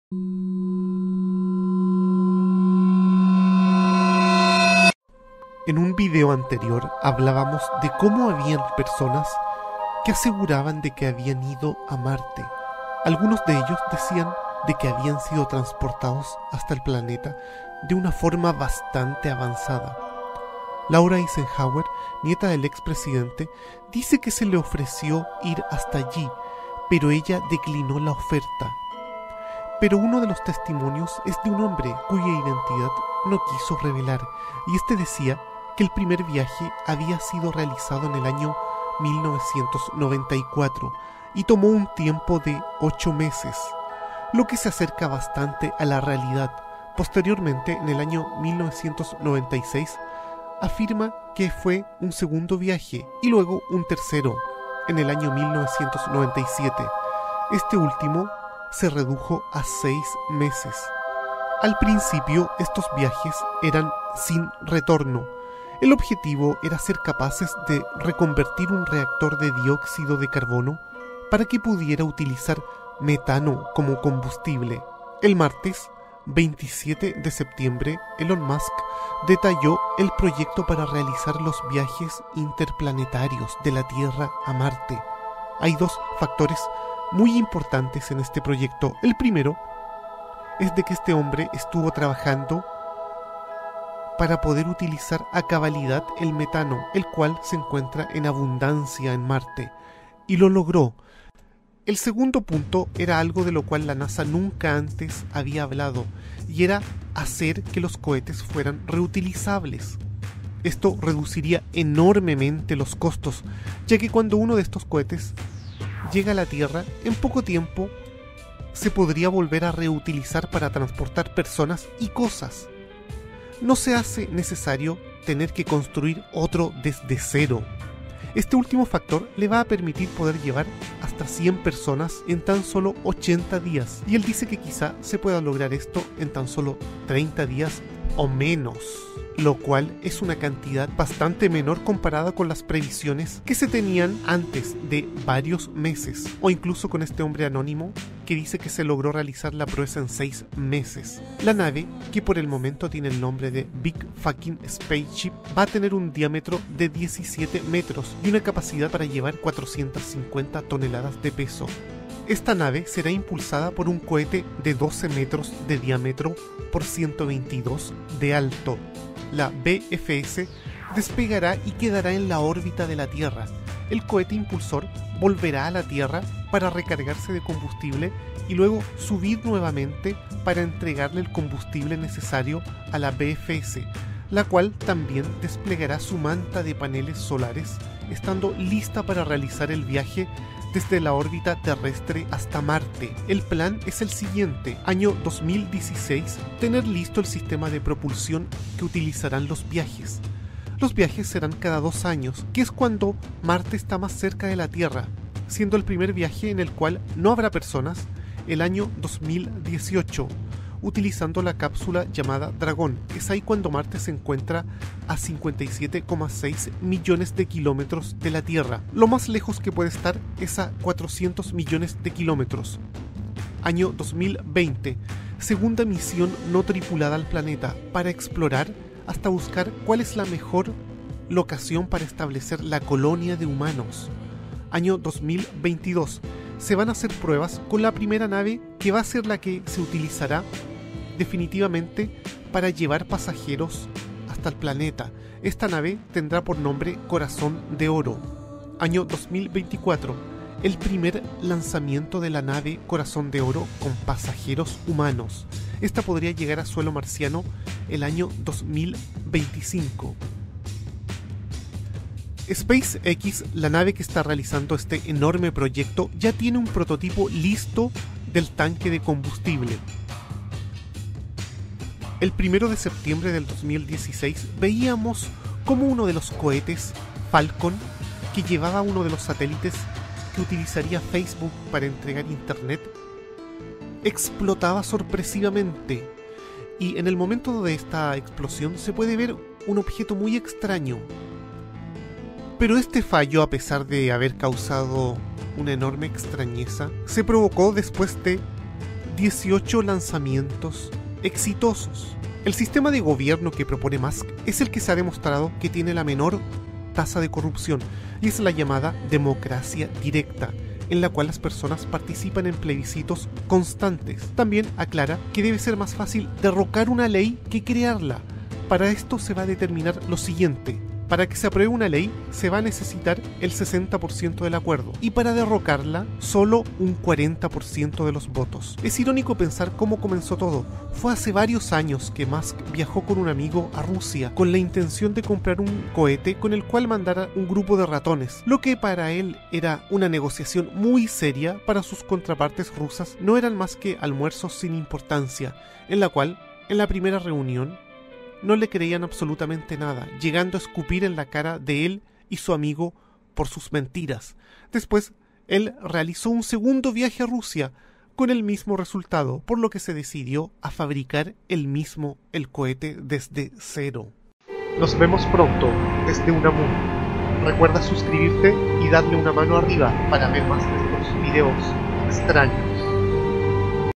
El Terrible Plan Para Llegar a Marte
El día martes 27  de septiembre de este 2016 en México Elon Musk detalló por primera vez como va a ser el plan maestro para ir a Marte, esto lo hace después de que el primero de septiembre un ovni pasara sobre el cohete Falcon justo cuando este explotara.